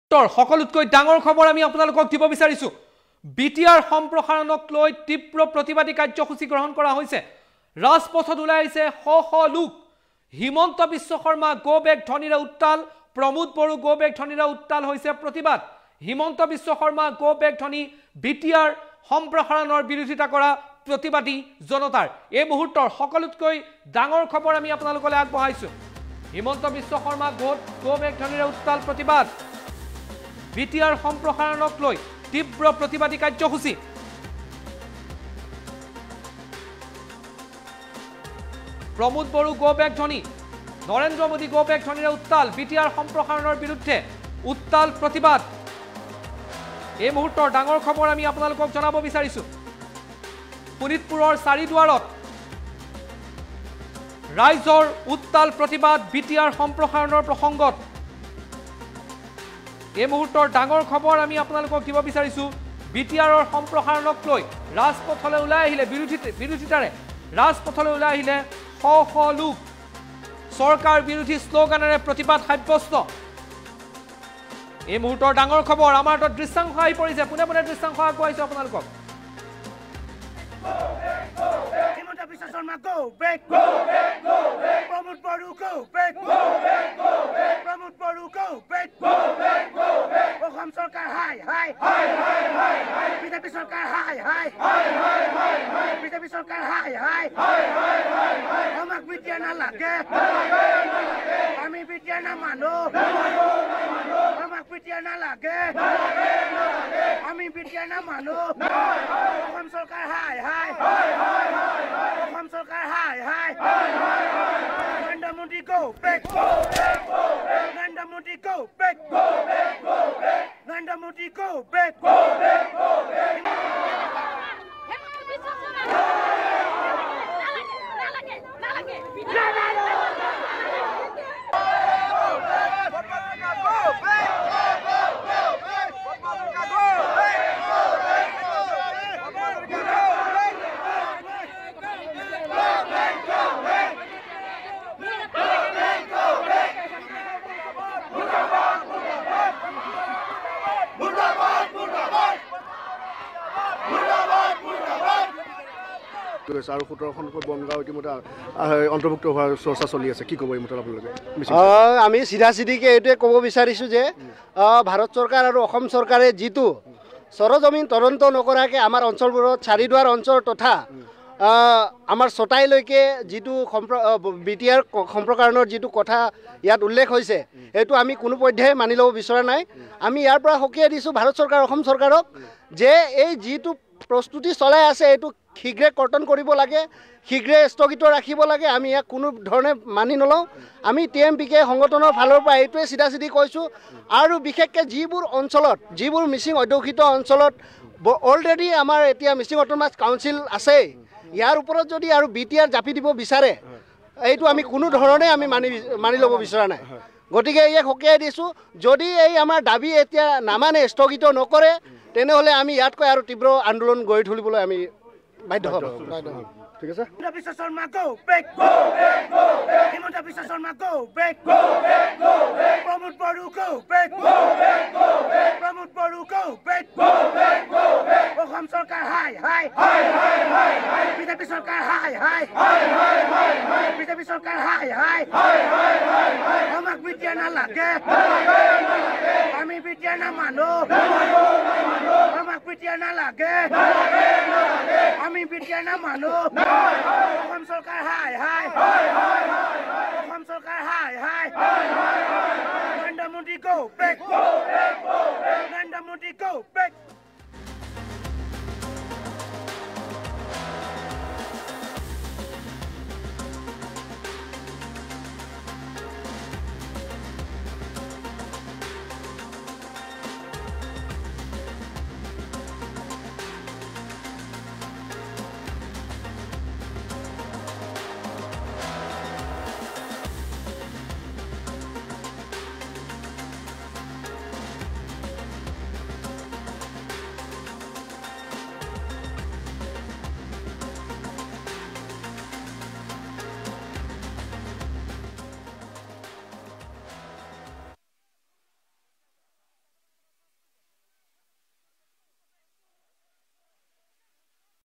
হিমন্ত বিশ্ব শৰ্মা গ' বেক ধ্বনিৰে উত্তাল প্ৰতিবাদ ।
প্ৰমোদ বড়ো গ' বেক, নৰেন্দ্ৰ মোদী গ' বেক ধ্বনি।
BTR সম্প্ৰসাৰণ কৰাৰ বিৰুদ্ধে উত্তাল প্ৰতিবাদ ।